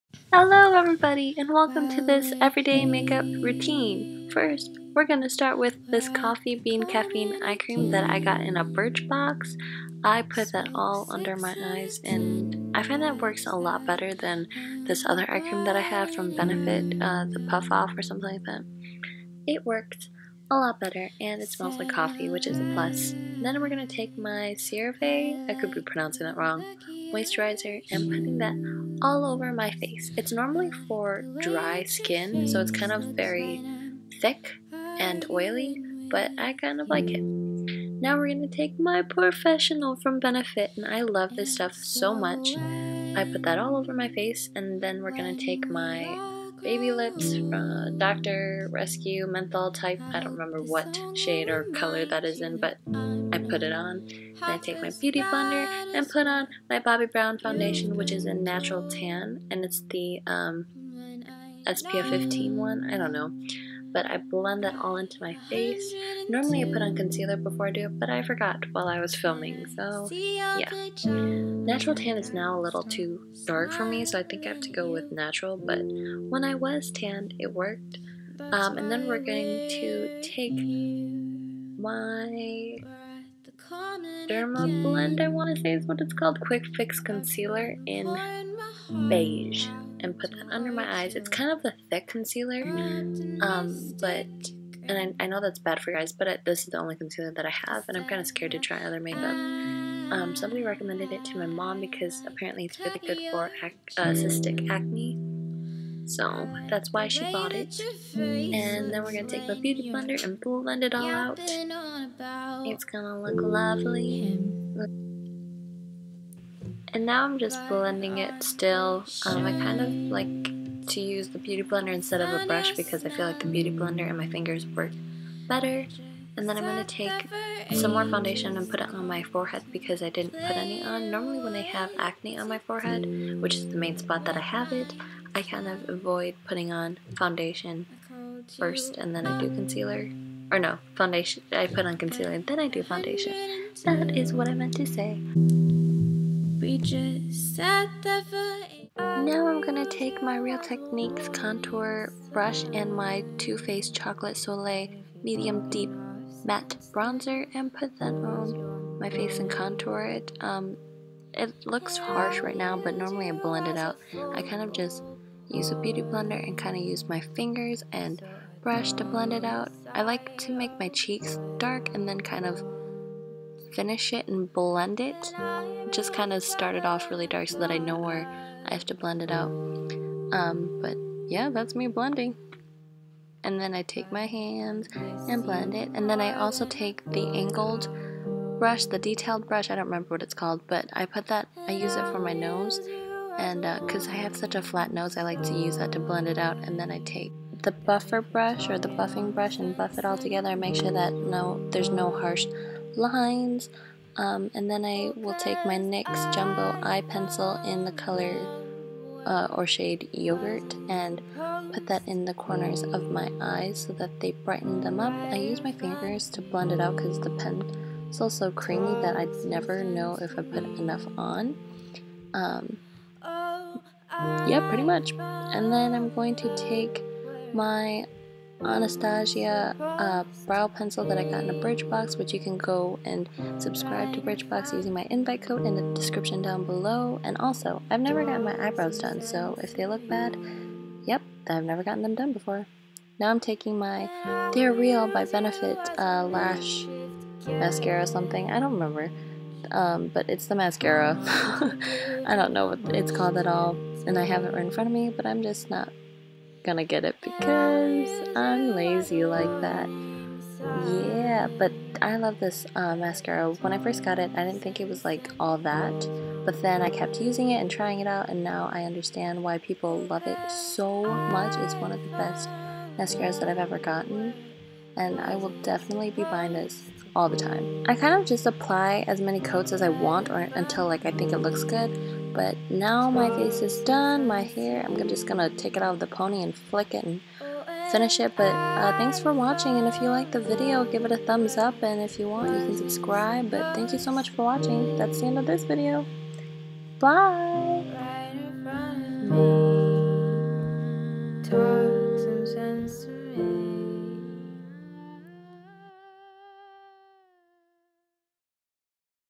Everyday Make-Up Routine Tutorial | Annika Cha
My EVERYDAY Make-up Routine!

Someone requested this in my last video! and I can't believe someone would actually want me to post this! 😅 BUT I did!! The background music is done by my friend Mary Sylph! So thank you Mary!!!

Products I Used:
100% Pure - Coffee Bean Caffeine Eye Cream
CeraVe Moisturizing Cream
The Porefessional
Bobbi Brown Skin Foundation SPF 15 - Natural Tan (They're the best foundation for yellow undertones)
Real Techniques Brushes
Too Faced Chocolate Soleil - Medium/Deep Bronzer
They're REAL Mascara
(not included in video, but when I wear LIQUID liner, this is the one I wear: Stila - JET Black
Anastasia Beverly Hills Eyebrow Pencil
Baby Lips Dr. Rescue ( I don't remember what shade. Either Pink Me Up or Berry Soft!)

LET ME KNOW IF YOU USE ANY OF THESE PRODUCTS AND IF YOU WANT ME TO REVIEW ANY OF THEM! 😁

SAY HI ON SOCIAL MEDIA!
Depop: acha4

Mercari Invite Code: RCMRBV
Uber Invite Code: uberannikac04